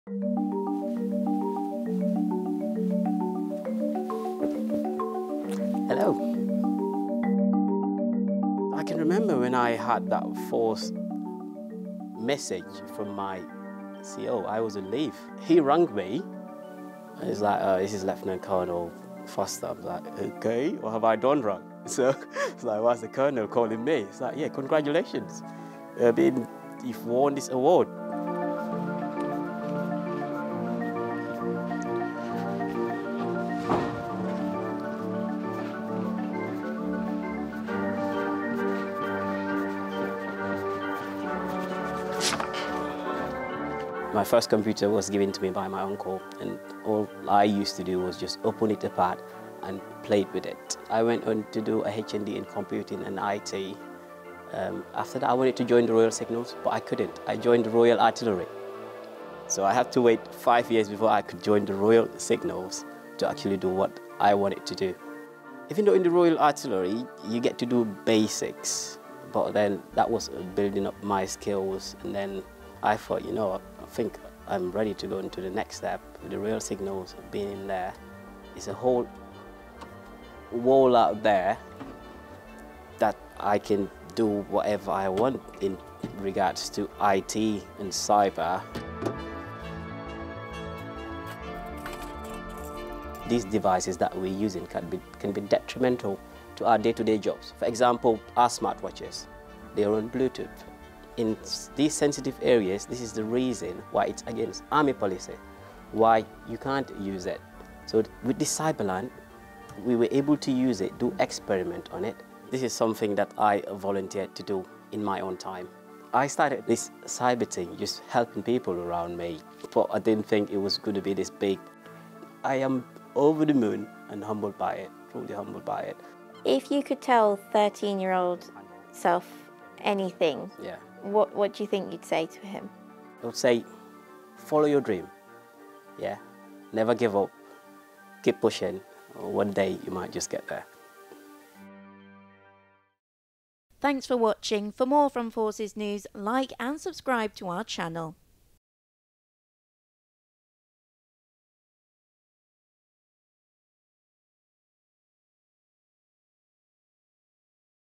British Army’s Royal Engineer of the Year passionate about cyber
An NCO from the Royal Corps of Signals has been telling Forces News how he helped nurture his fellow soldiers' interest in cyber.

Corporal Pa Malick Adams, from 16 Signal Regiment, recently won the Royal Engineer of the Year award after his efforts were recognised by the Worshipful Company of Engineers.

His passion for cyber, along with his desire to help develop his colleagues' knowledge of cyberspace, led him to create a facility where both novices and experts can safely experiment with offensive and defensive cyber scenarios.